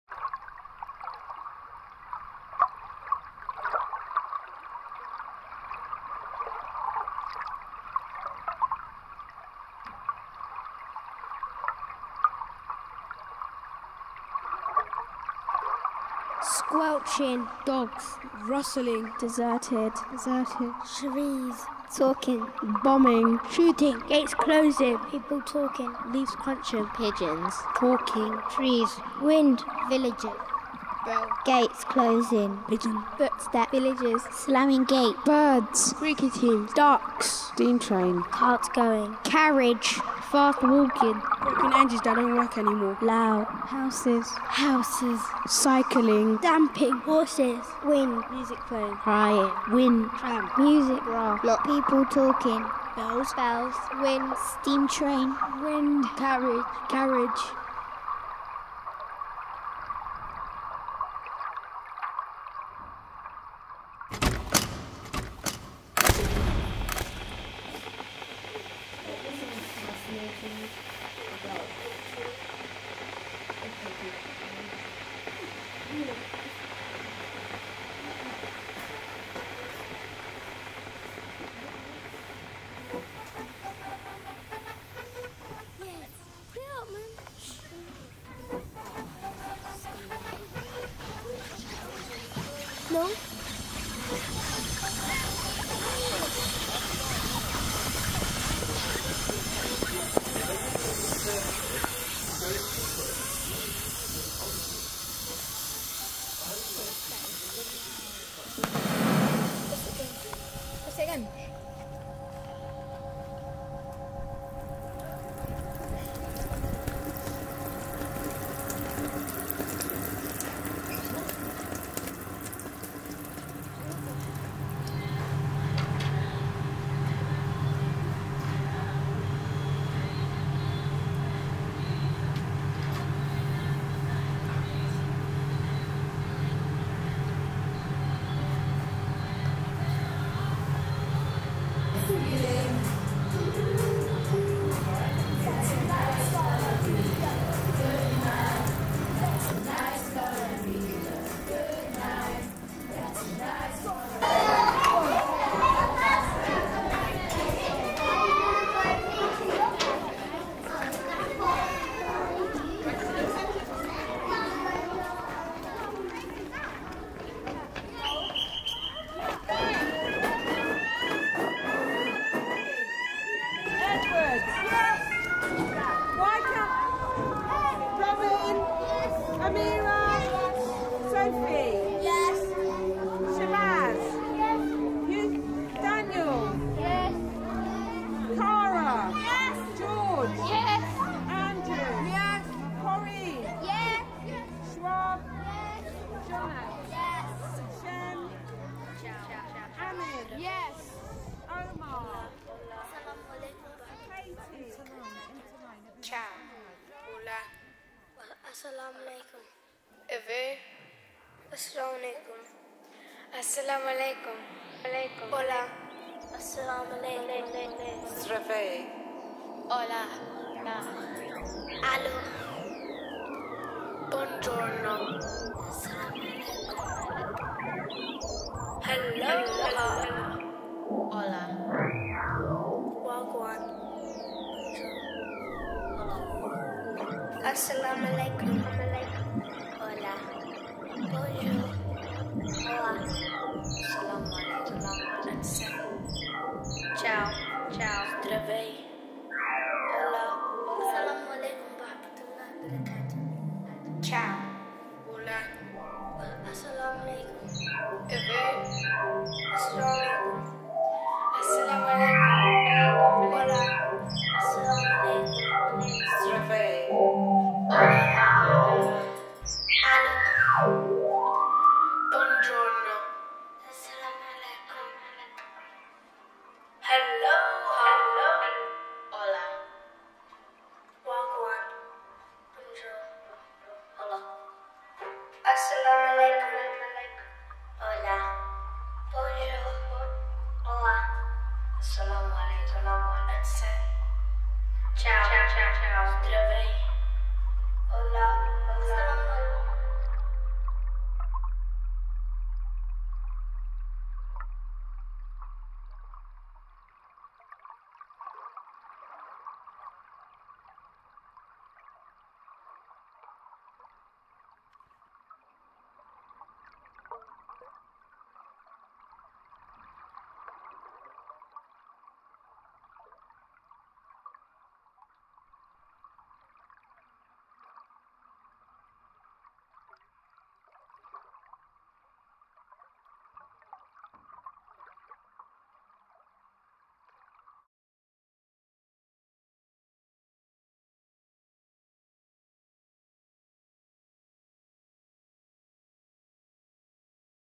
Woodmansterne School
Sounding It Out, was a project led by composer Duncan Chapman with Lambeth CLC , working in 5 Lambeth primary schools to create sound pieces that explored notions of past,present and future soundworlds in the local environment. The images are some that were used as stimulus together with images of sounds collected which will form part of an interactive installation in a new building in summer 2010